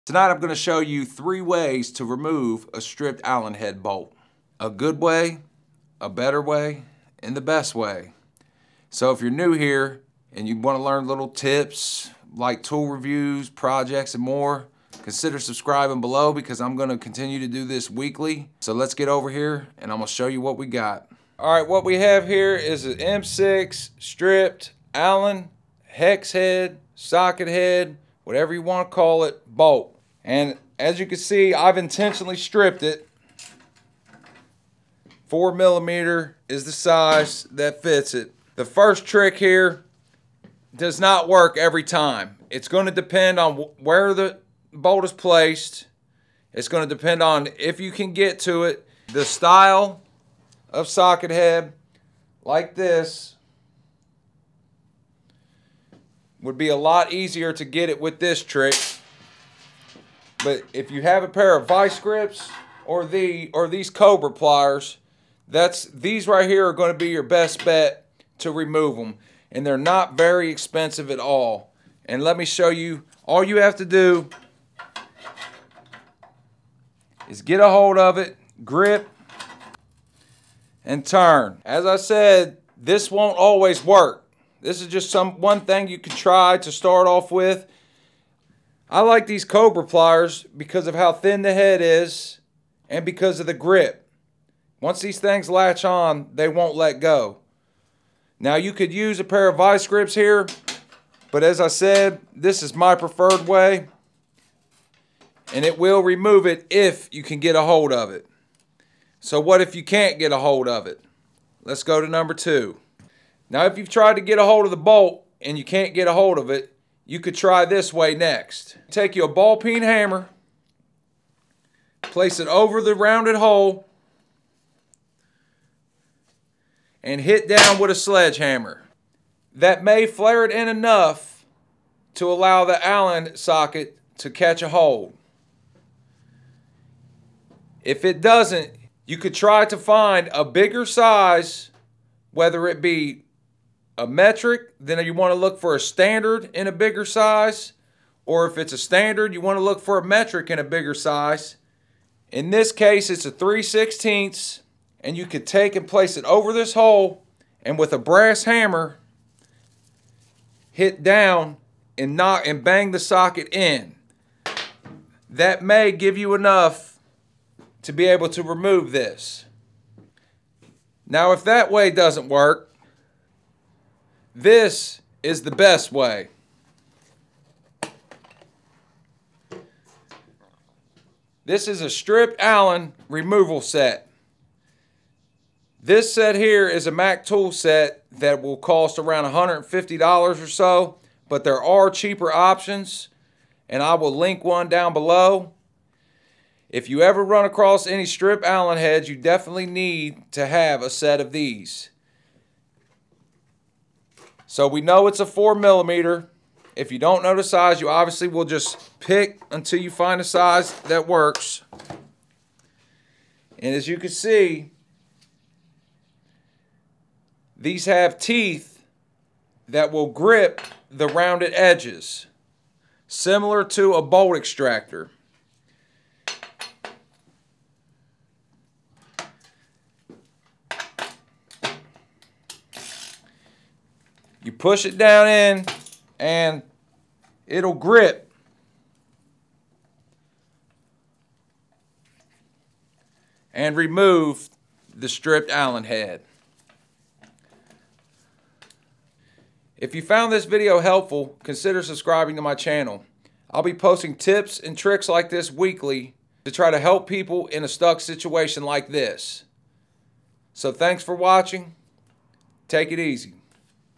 3 Easy Tricks to Remove Stripped Allen Head Bolts
Going over 3 quick and easy ways to remove a stripped allen head bolt.

Allen Socket Remover Set

10 inch Knipex Cobra Pliers

3 set of Knipex Cobra Pliers

Small Knipex Cobra Pliers